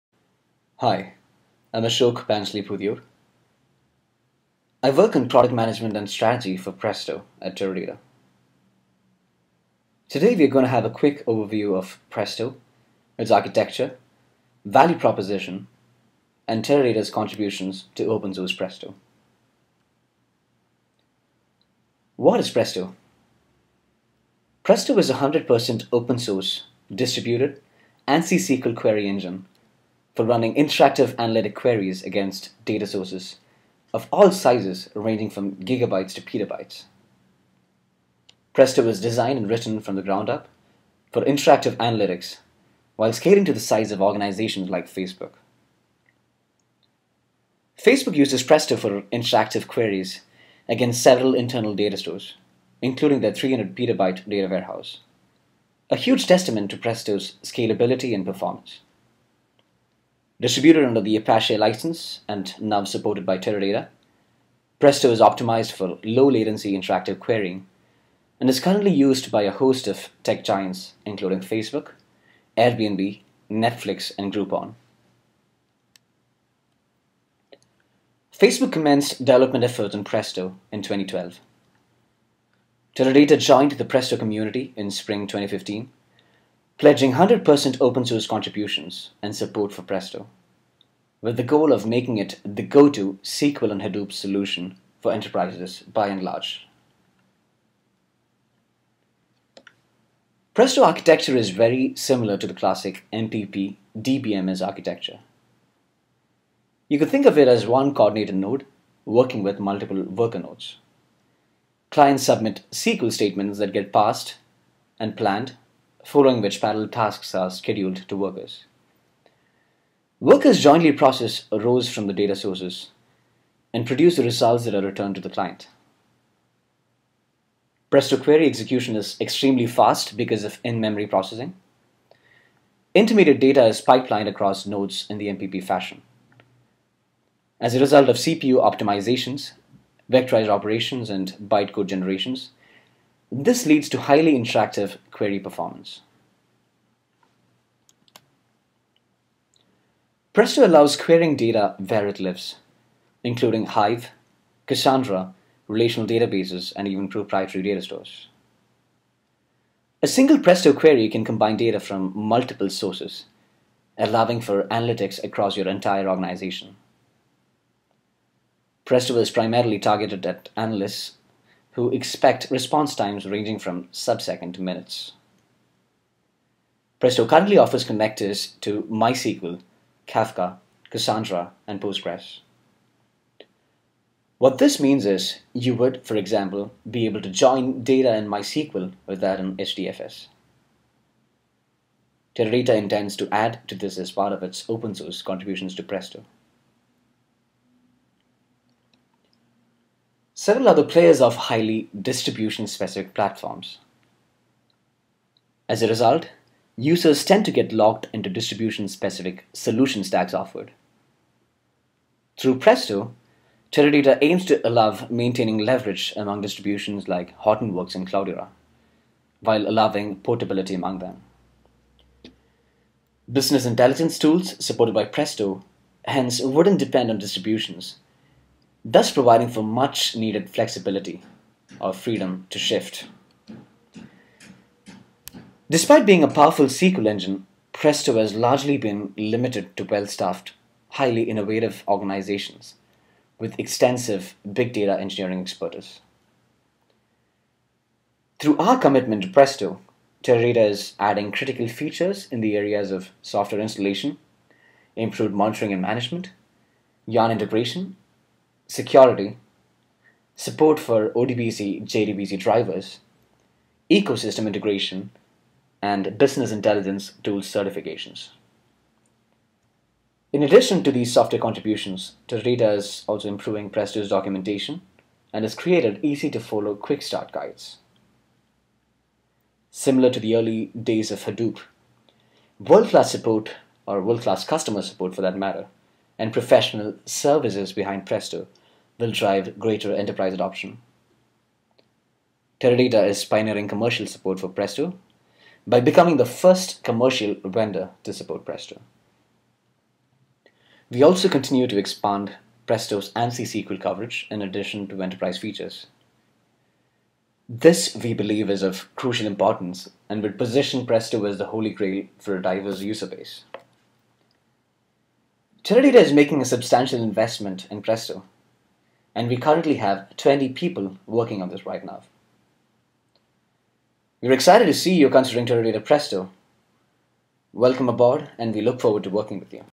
Presto Overview Video
This quick 5 minute video will provide an overview of the open source Presto SQL on Hadoop query engine. The video highlights its functionality and architecture of Presto as well as the Teradata contribution to the Presto  roadmap.